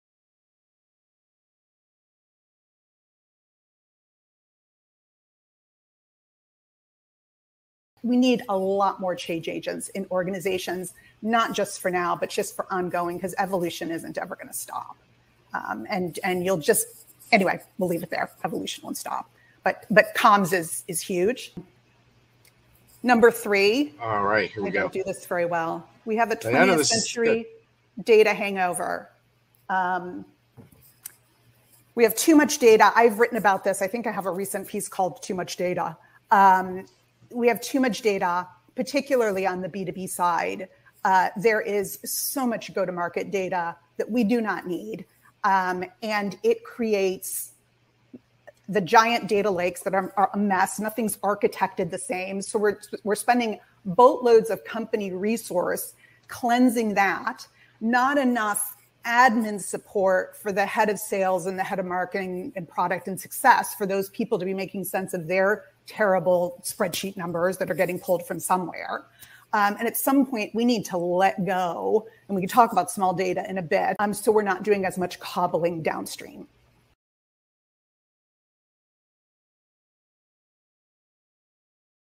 Breaking free from data cobbling - Maureen Blandford on the barriers to enterprise data insights
In this short highlight from our recent video show on getting real about enterprise data - and why digital transformations fail - Maureen Blandford of Serendipitus reveals one of her top reasons why digital transformations fail:  wait, we have too much data, particularly on the B2B side? In this clip, Maureen explains: giant data lakes are a mess. Oh, and people are struggling to make sense of their "terrible spreadsheet numbers" - ouch! That's a recipe for a 20th century data hangover.

Full show replay on LinkedIn Audio-only podcast version